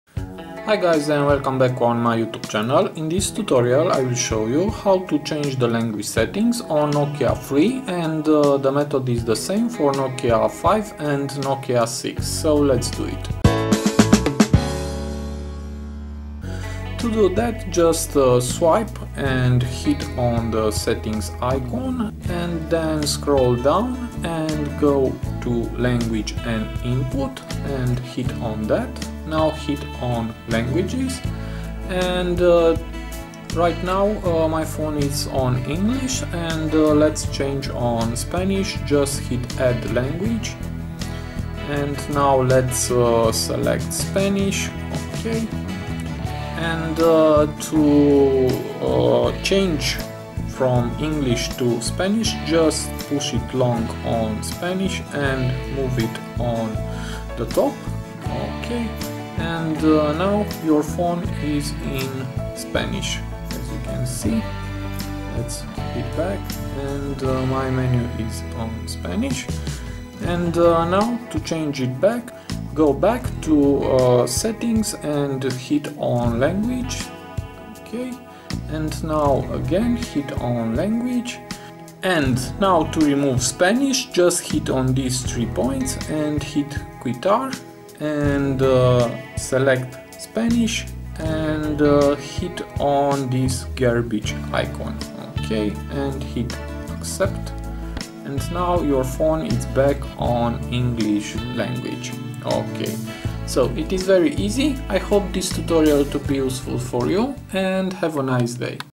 NOKIA 3, 5, 6 (2017) - How to Change Language Settings
In this video tutorial I will show you how to change language settings on Nokia 3, 5, 6 (Android 7.0) by using phone settings.